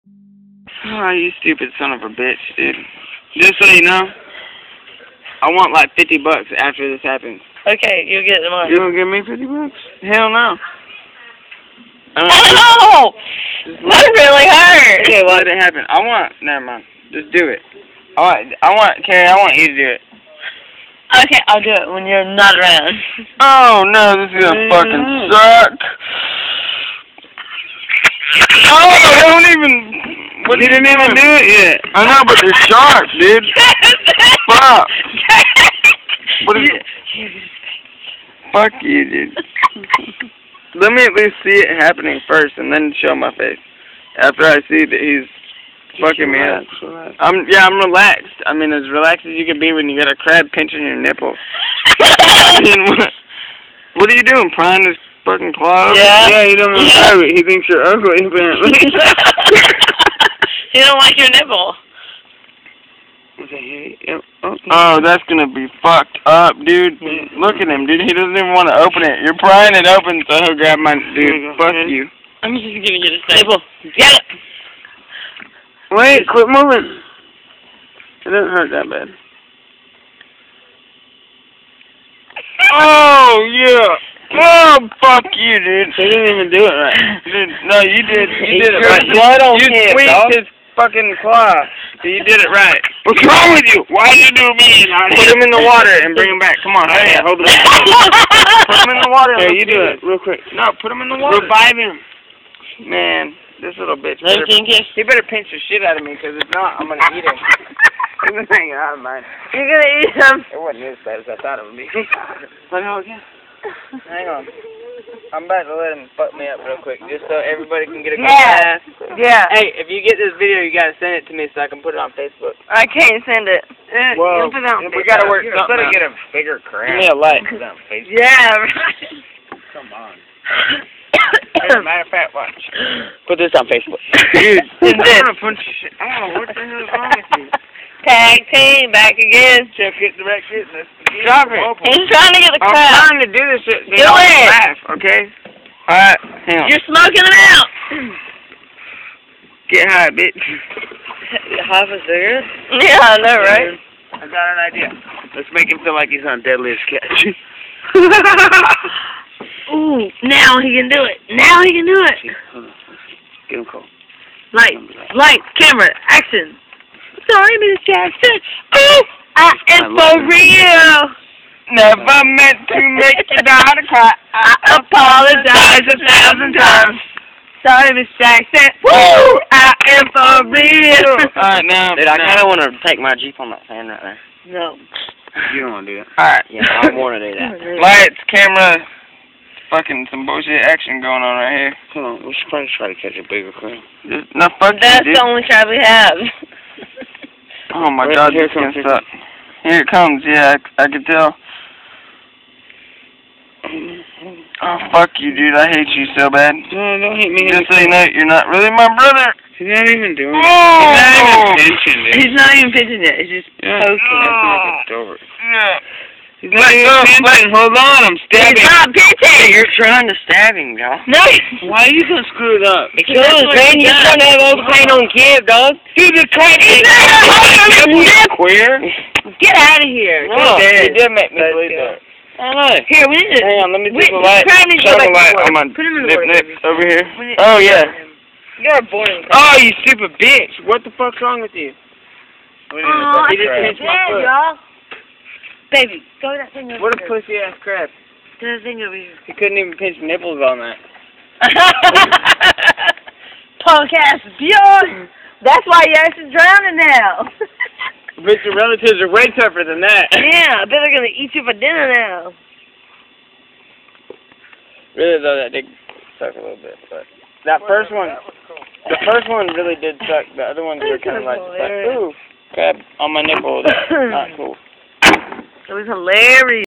Drunk people!
Kevin getting his nipple pinched!!